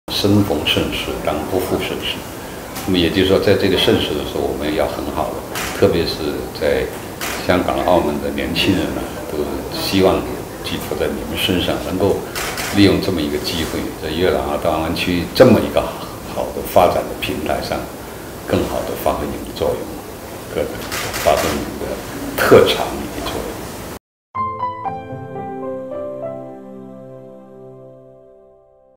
钟南山寄语港澳青年：生逢盛世当不负盛世
4月20日，钟南山院士在广州接受记者采访时表示，粤港澳大湾区是我国开放程度最高、经济活力最强劲的地区，也是很多年轻人逐梦的地方。钟南山院士给港澳青年送出青春寄语，同时也是他在全国抗击新冠肺炎疫情表彰大会上讲过的一句话：生逢盛世，当不负盛世。钟南山院士认为，希望在年轻人身上，他们在粤港澳大湾区这样好的发展平台上，应该更好地发挥作用。